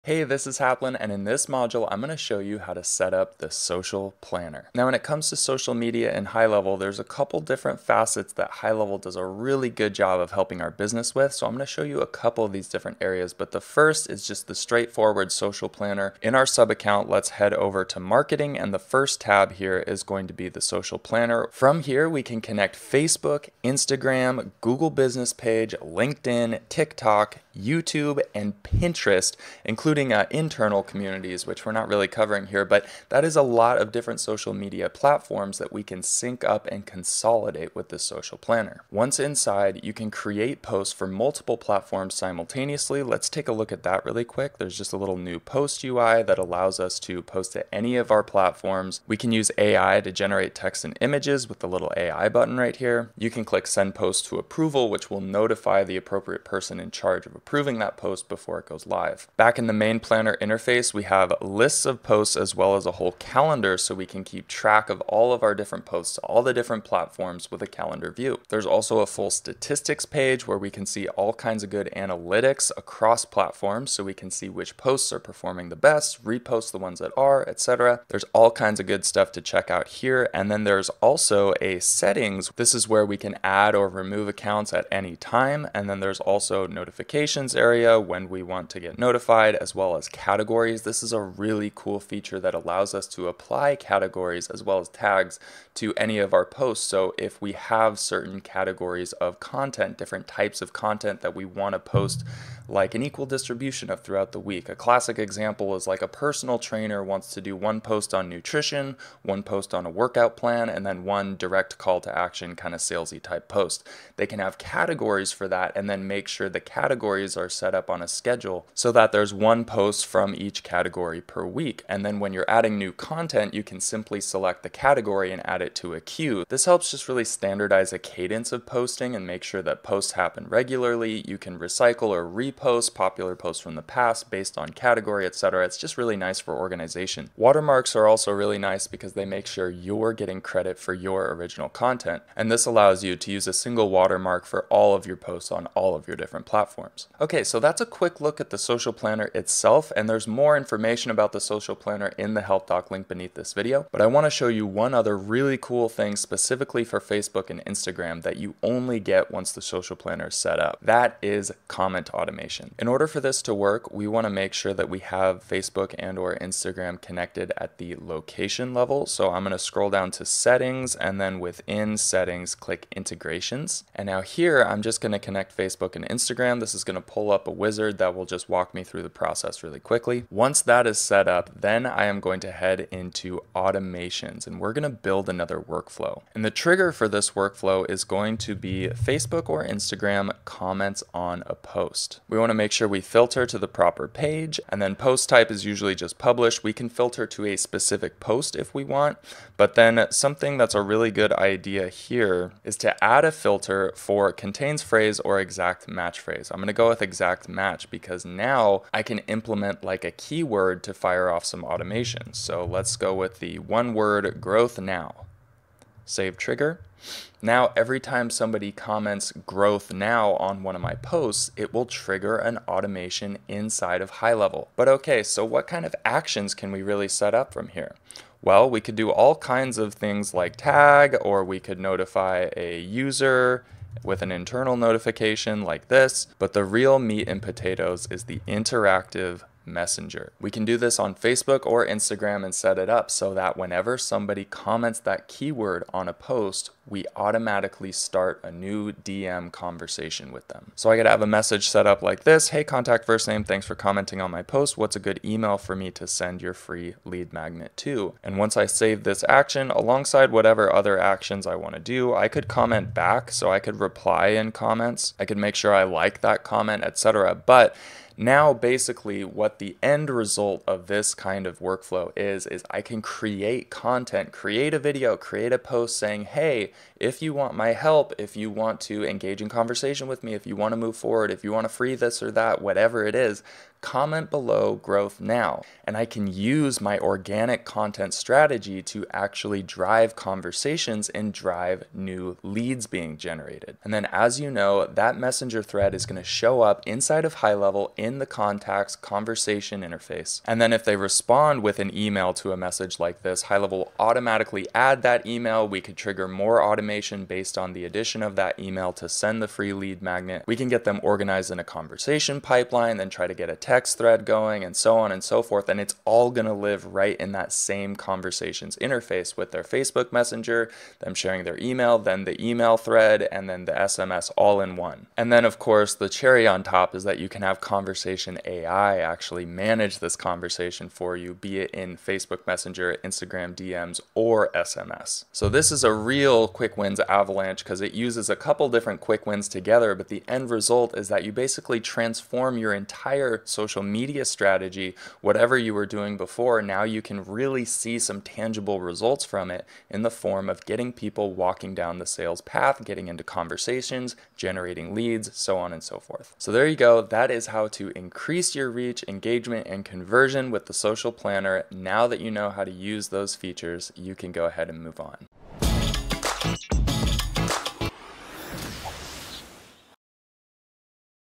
🏙️ Increase Your Reach With The Social Planner
Add the Quick Wins 2025 Snapshot into your HighLevel account by clicking

Ultimate Guide to HighLevel Social Planner: Boost Your Social Media Strategy!

Join Haplin as he walks you through setting up the powerful HighLevel social planner. Discover how to connect and manage multiple social media platforms like Facebook, Instagram, LinkedIn, and more. Learn to create and schedule posts, utilize AI-generated content, monitor analytics, and apply categories to streamline your social media efforts. Plus, get an in-depth look at comment automation for Facebook and Instagram, enabling you to drive engagement and generate leads effortlessly. Transform your social media strategy and see tangible results today!